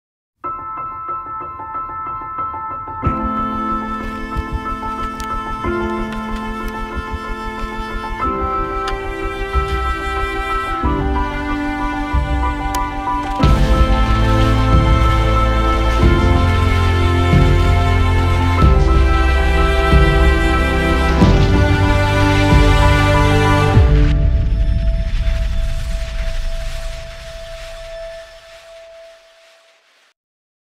MATTIA FELICETTI - Freeride Ski
Mattia Felicetti - Italian Freerider exploring the Dolomites

Filmed and Edited by: Federico Modica